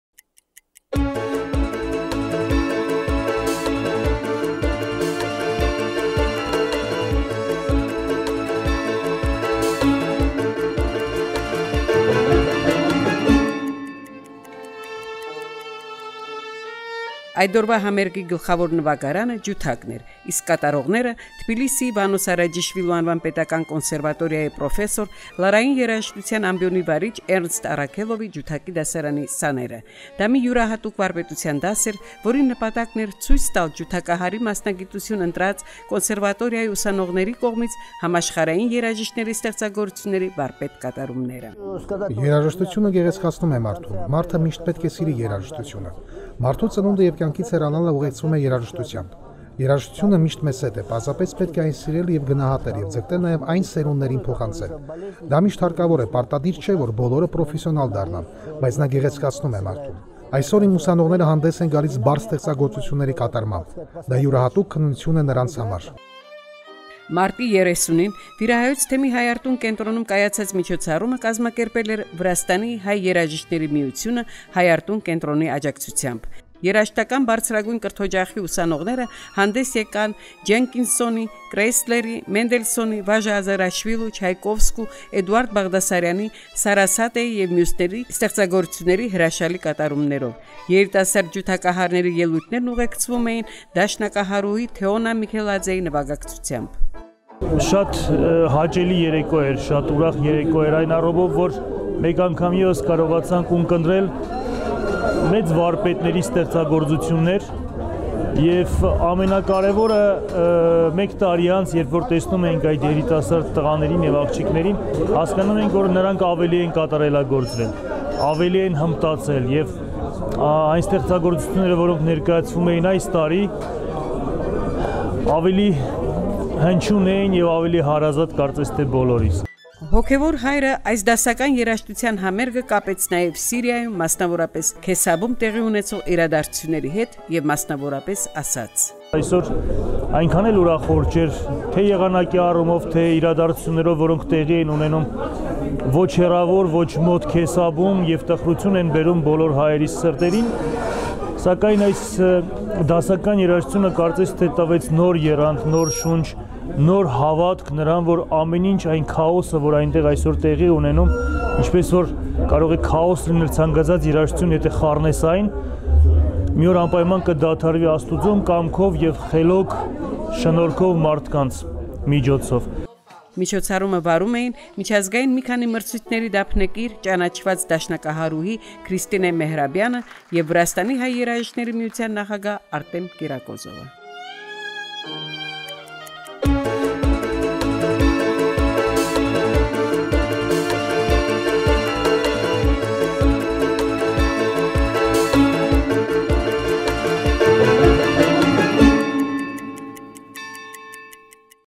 Мастер-класс профессора Эрнеста Аракелова с участием студентов консерватории
Պրոֆեսոր Էռնստ Առաքելովի վարպետության դասը կոնսերվատորիայի ուսանողների մասնակցությամբ

პროფესორ ერნსტ არაქელოვის მასტერ-კლასი კონსერვატორიის სტუდენტების მონაწილეობით

Workshop with Professor Ernest Arakelov featuring the conservatory students